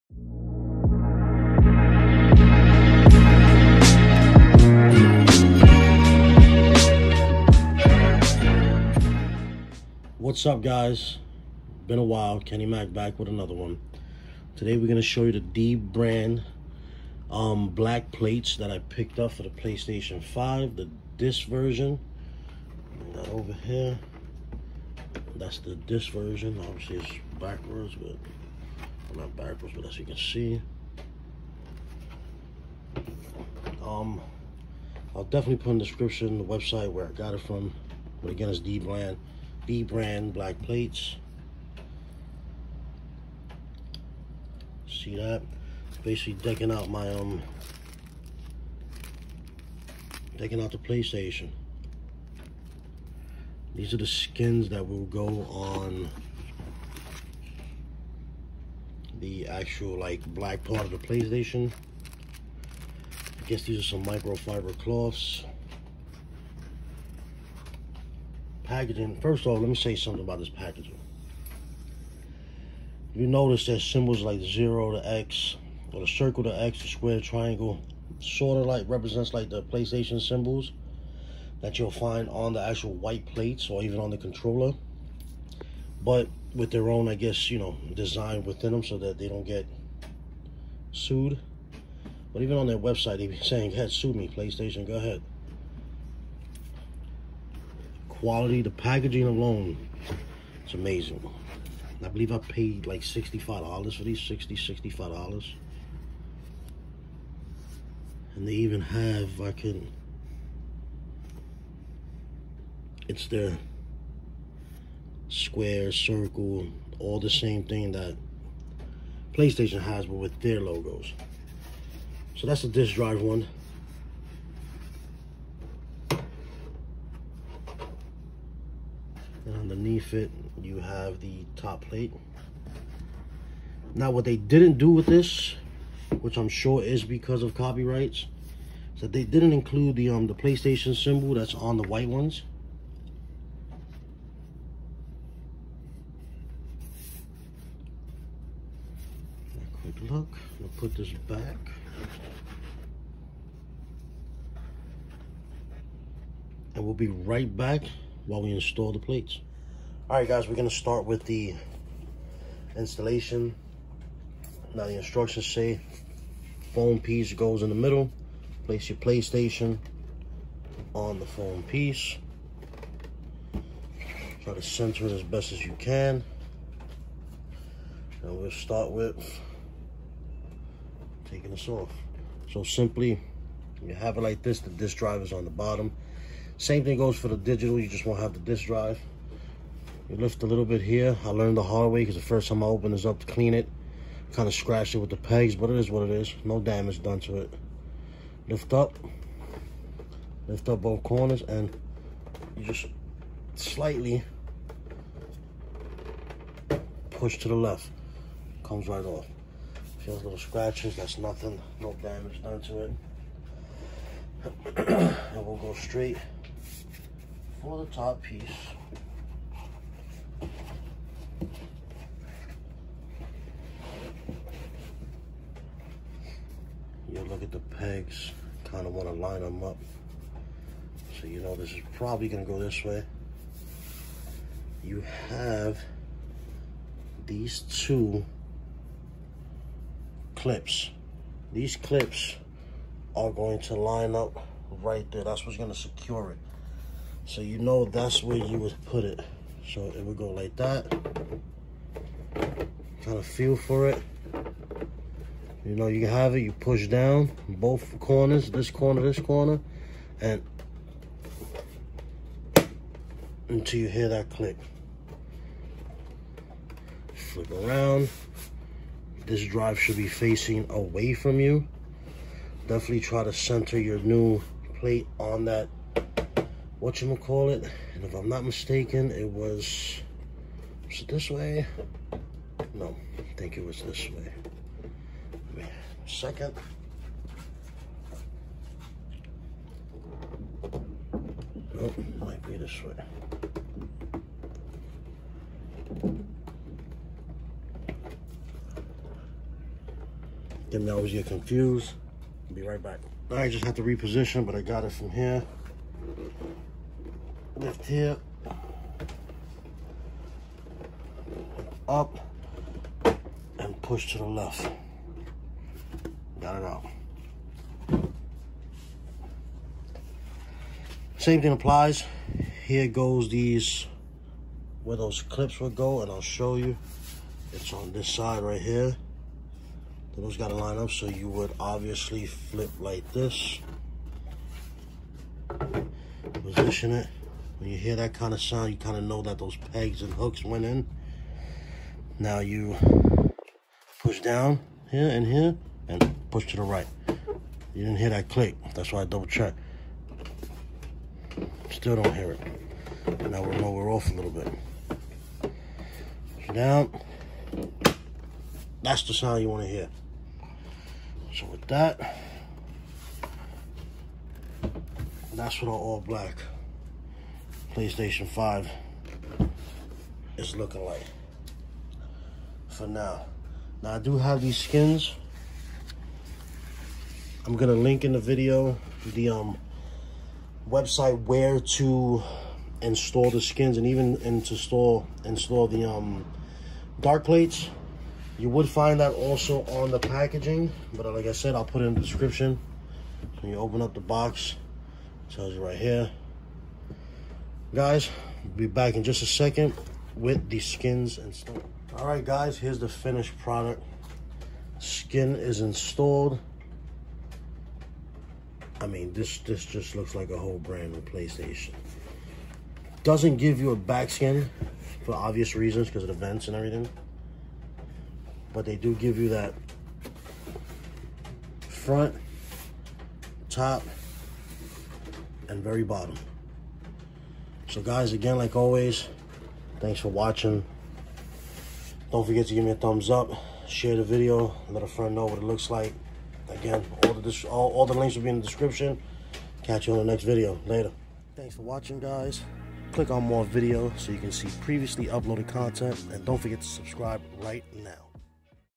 dbrand Darkplates For The Ps5 Disc Version
darand Darkplates installation on the PS5 disc version

Follow My Partner The Video Game Votary!